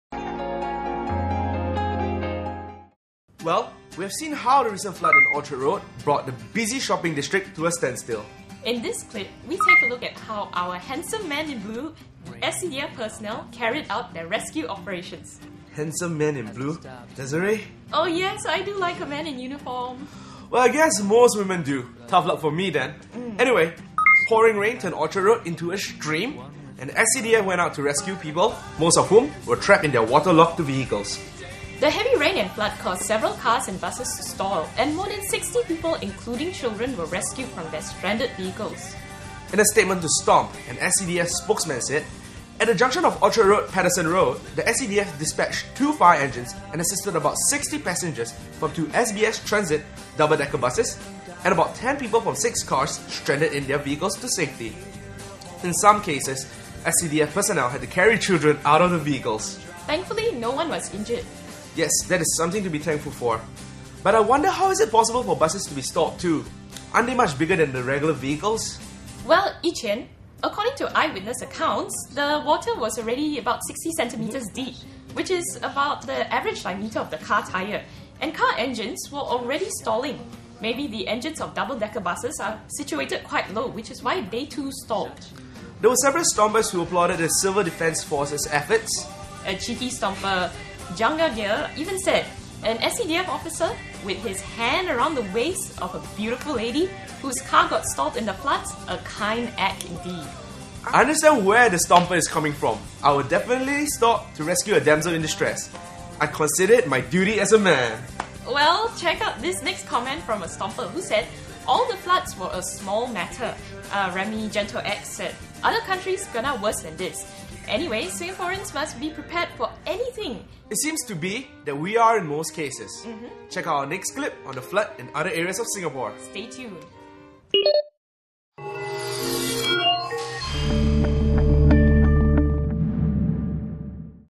SCDF and the flood (Floods in Singapore Pt 3)
We applaud the heroic rescue work by the SCDF during the floods.